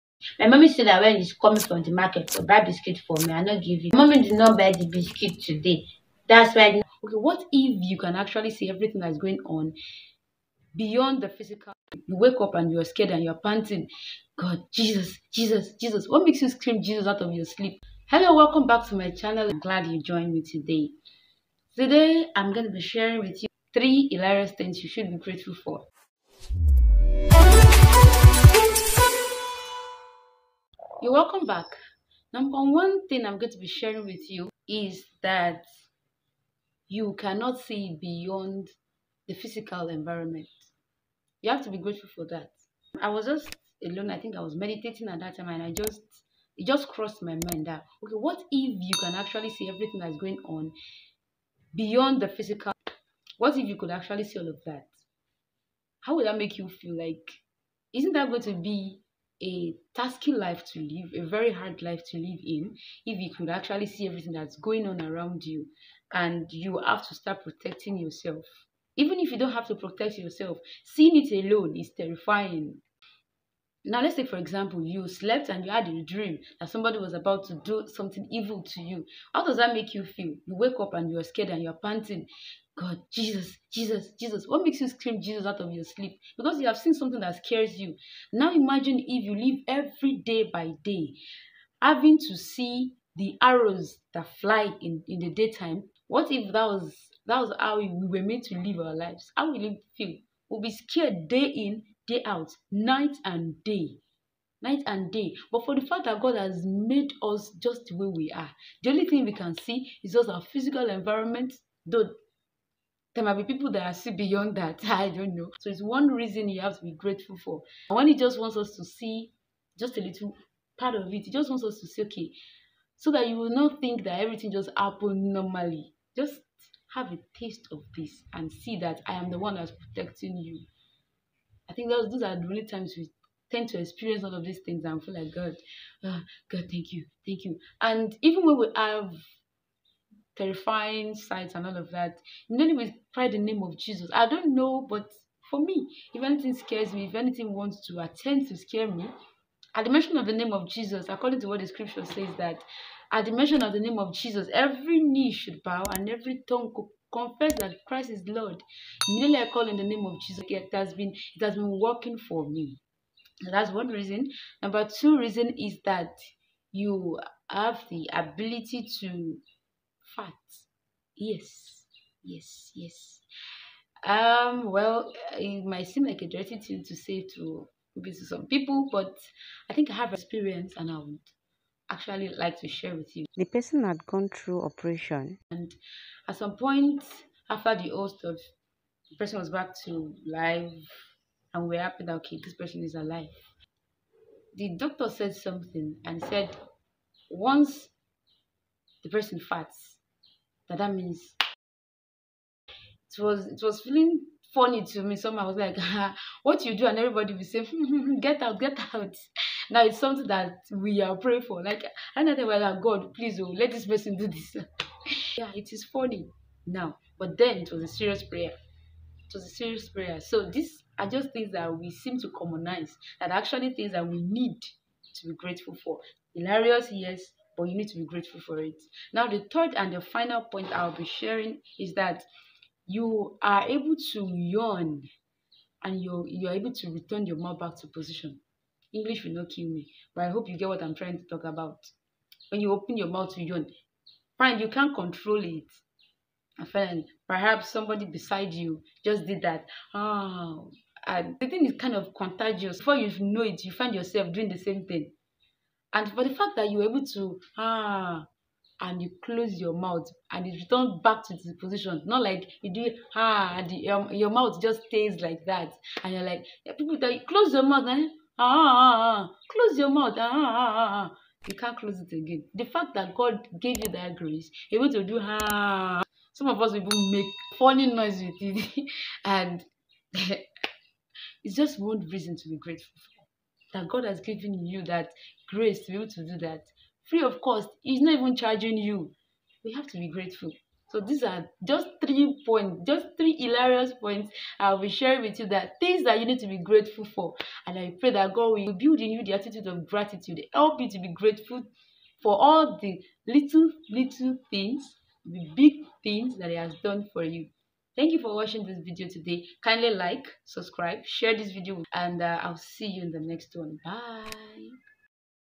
3 Hilarious Gratitude Points
We have so undermine many blessings we have received from God. Because they happen in the most natural way, we tend to see them as norms.

Here are 3 hilarious gratitude points. I hope you find reasons to be grateful in place of feeling entitled to this 'natural blessings.'